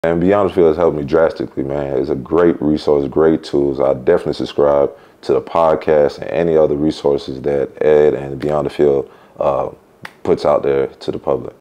Beyond The Field Testimonial - Rod Rook-Chungong, Director of Player Development - GSU Football
Rod Rook-Chungong, Director of Player Development & Community Engagement for Georgia State Football, gives a great testimonial to the resources @btf_program provides.

Rod used the resources to help him secure his position as a player development director.

It's your turn! The resources can be found below.